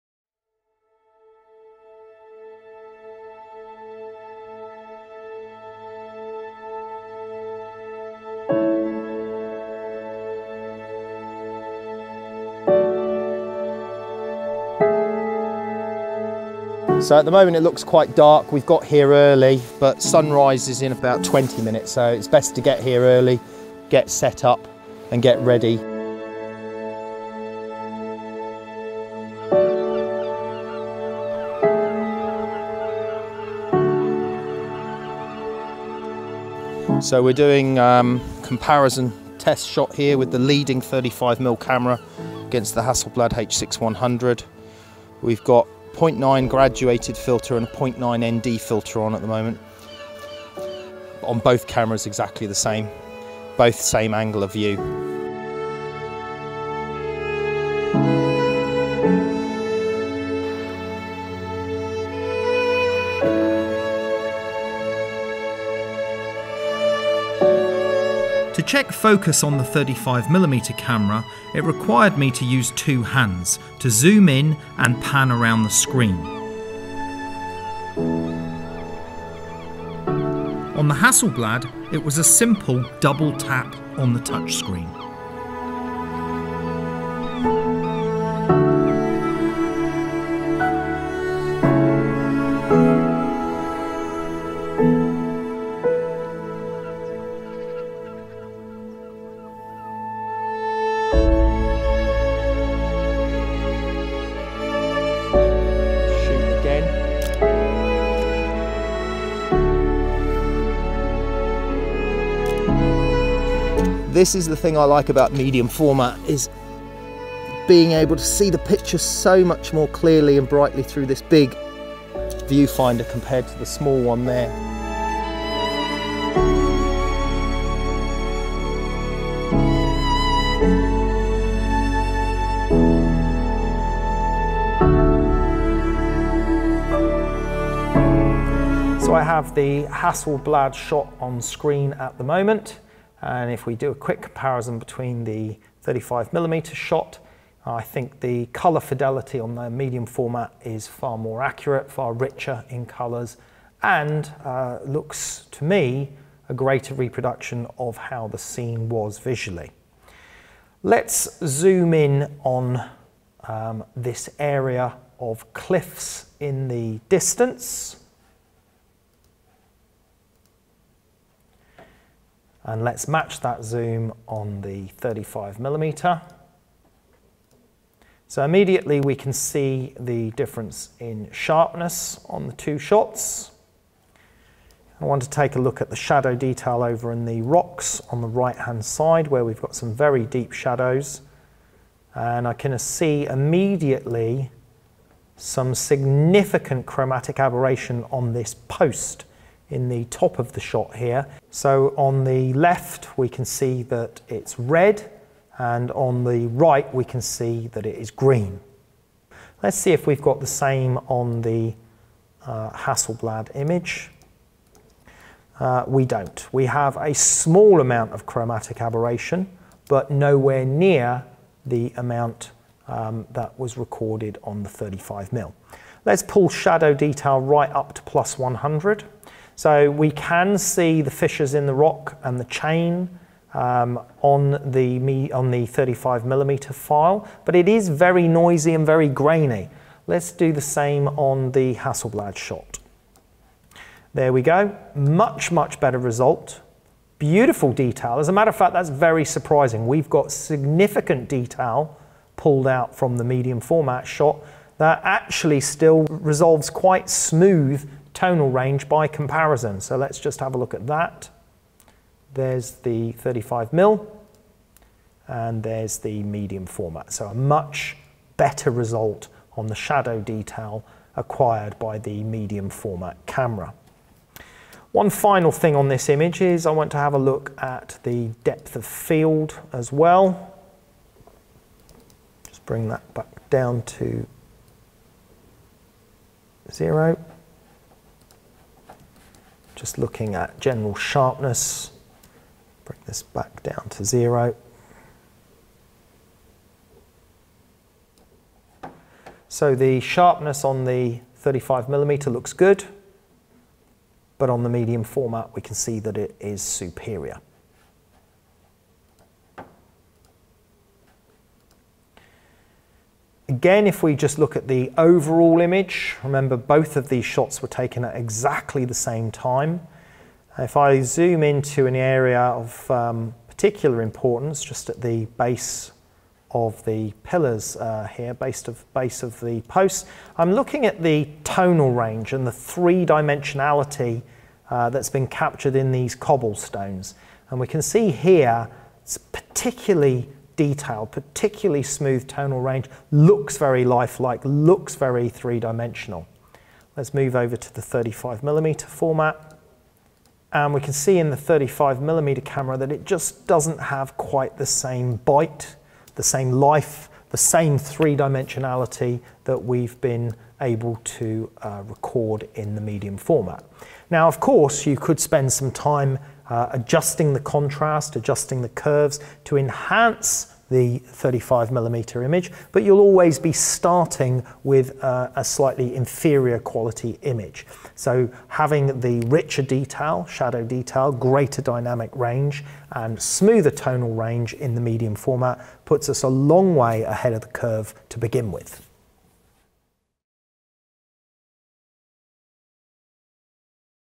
Medium Format Advantages, Sunrise Captures and Image Comparison
This series examines the advantages of Hasselblad medium format over the leading 35mm format camera system. Photographer and technical expert, Karl Taylor puts the Hasselblad H6D-100c to the test while capturing the sunrise with both cameras.